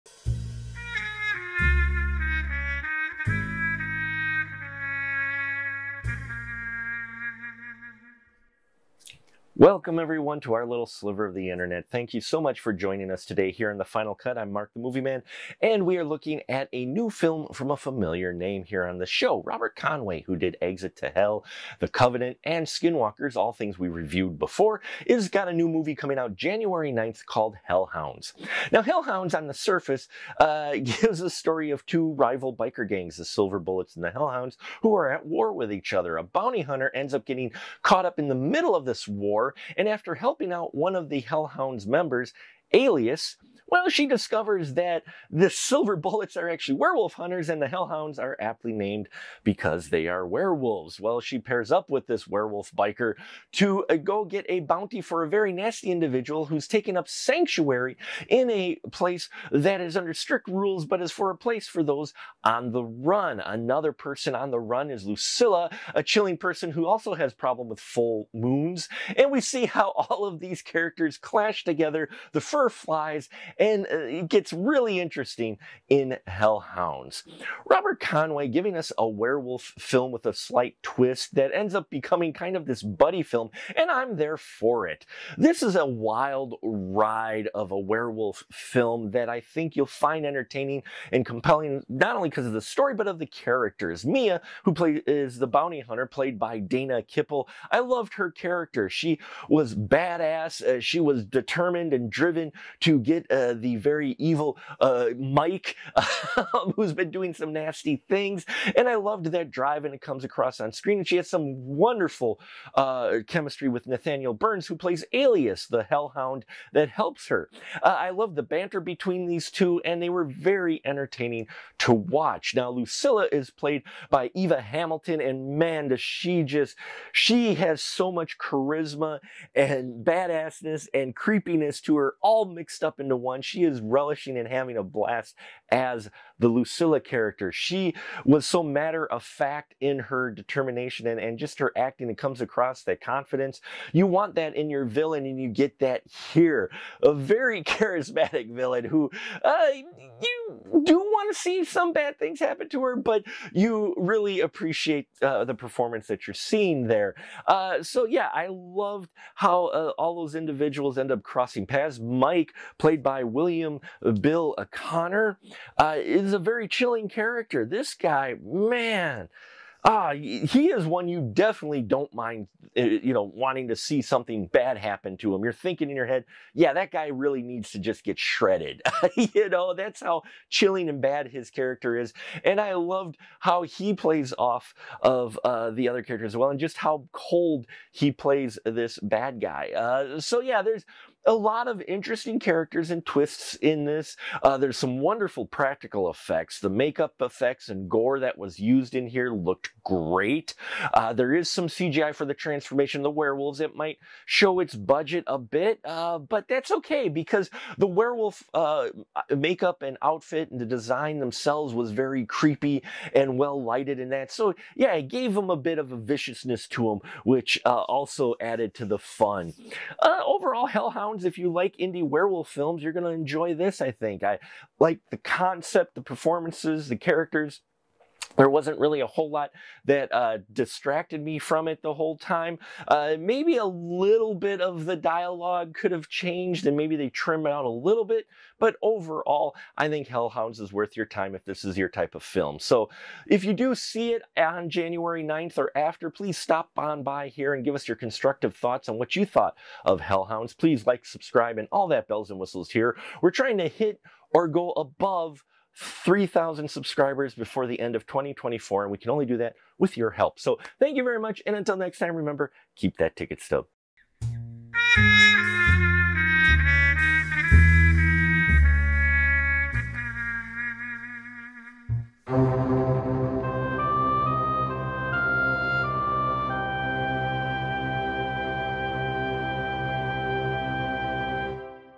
Hellhounds (2024) moviereview review - The Final Cut
We ride out with our review on a new feature film from a familiar name. We look at Robert Conway's Werewolf horror "Hellhounds".